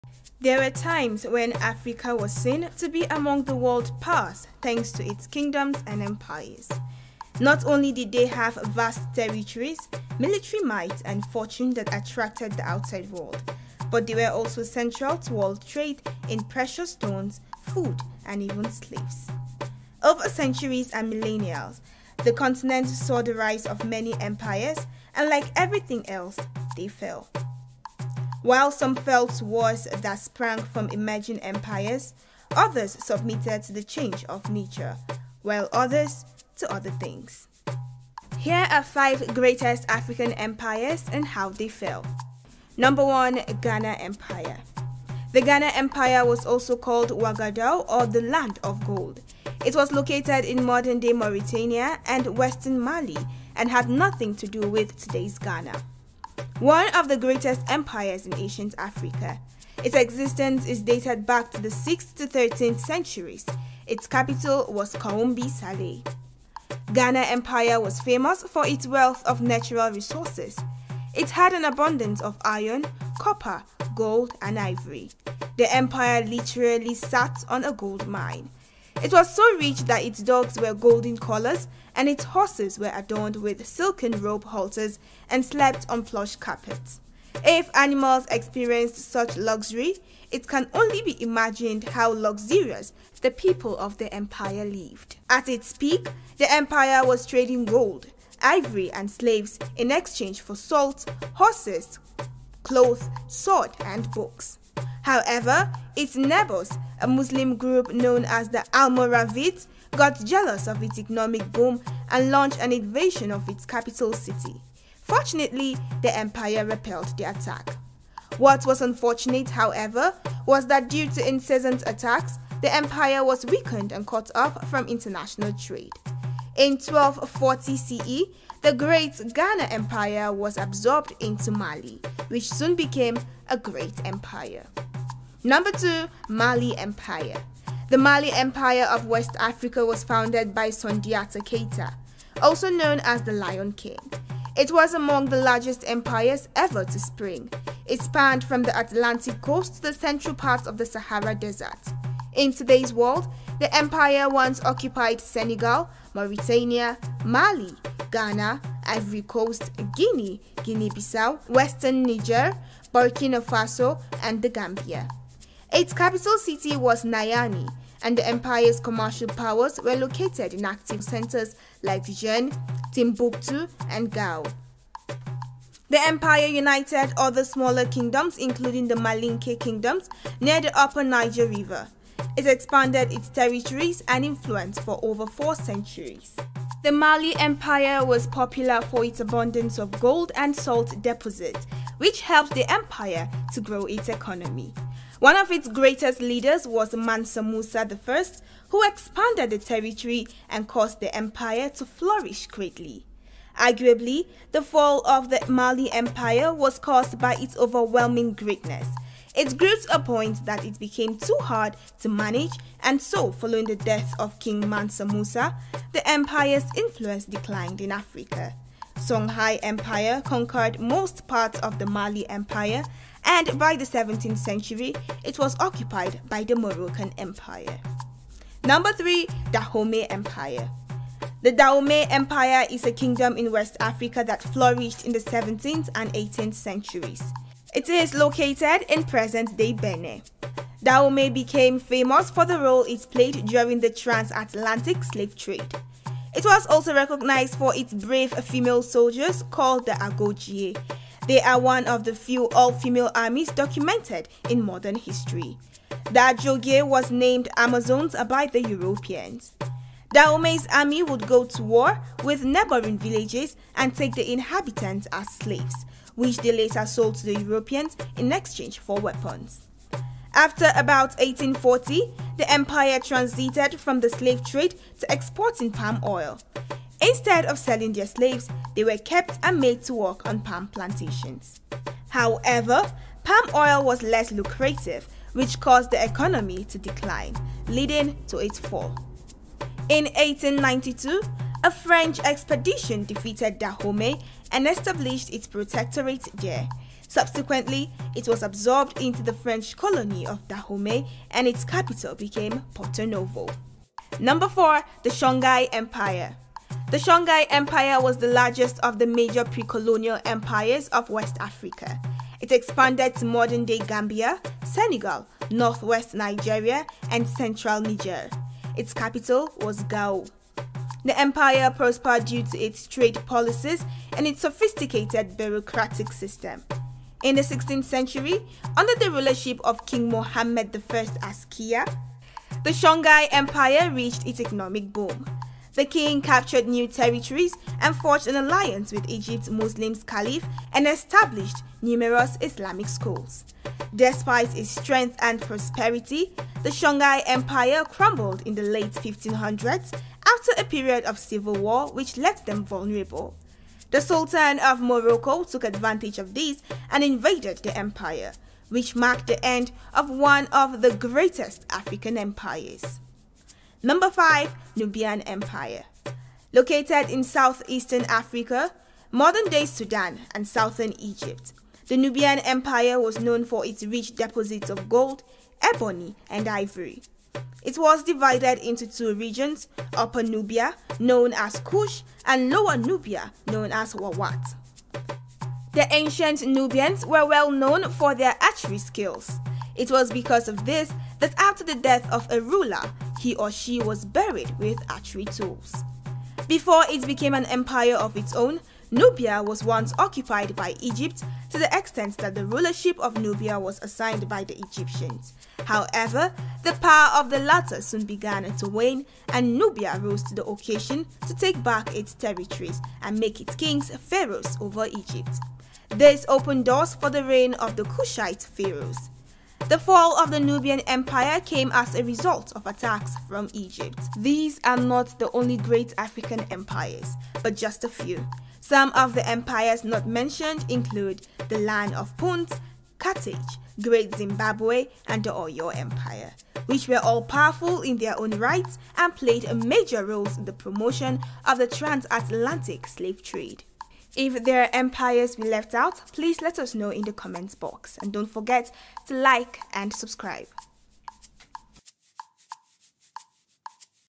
5 Greatest African Empires: Truth About How They Rose And Fell
There were many great African Kingdoms and empires in the past. Here are 5 among the greatest empires in ancient Africa, with a look at how they started and fell.